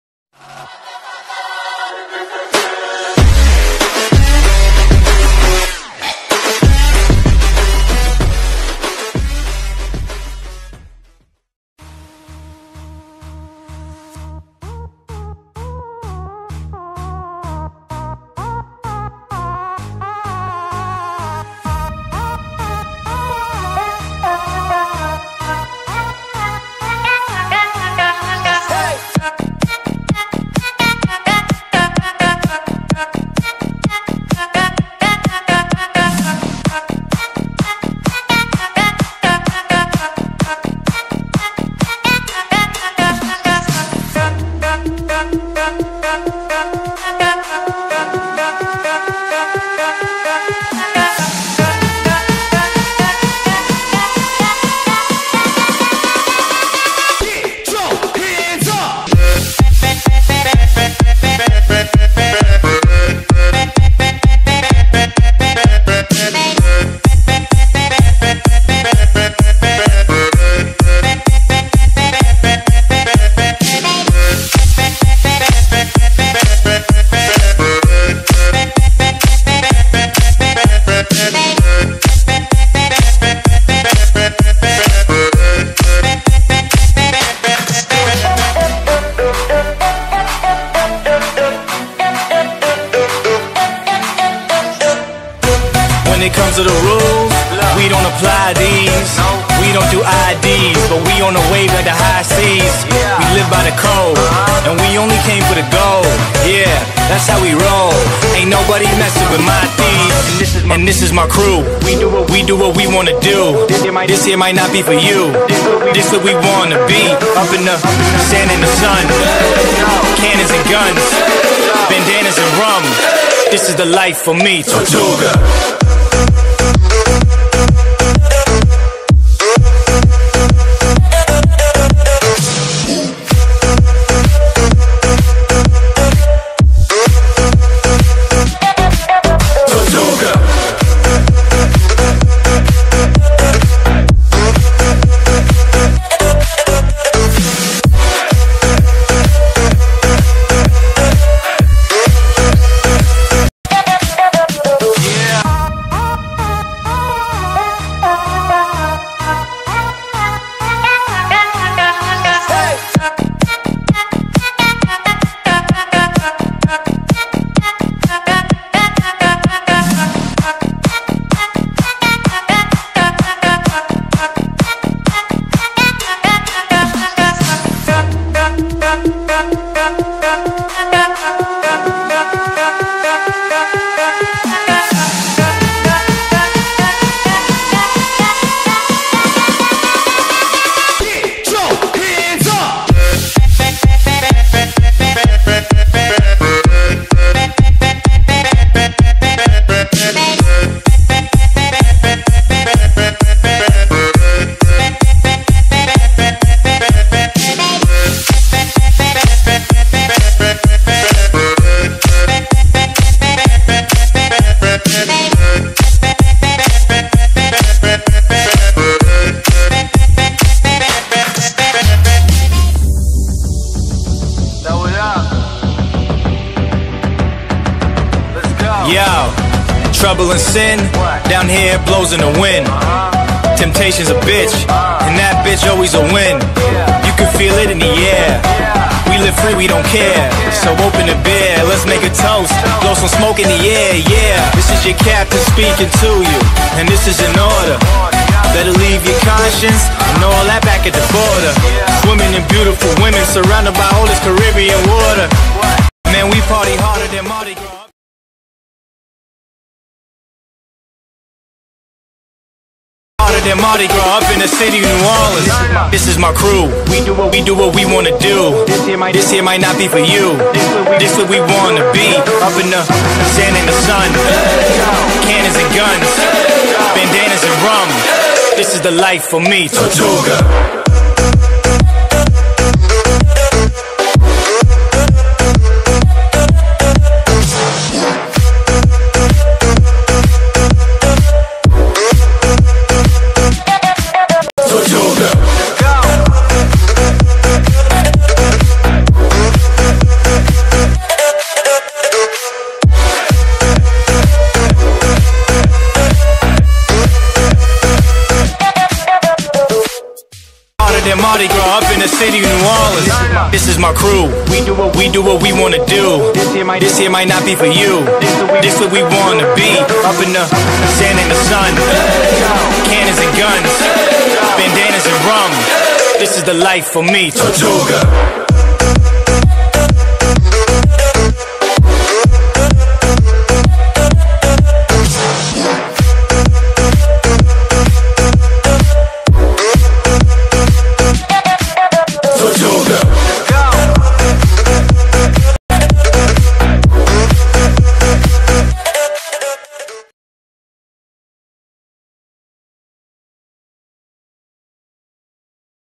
Changing colour of a picture using |Snapseed| and adding your logo using |PicsArt
Song: car electro House mix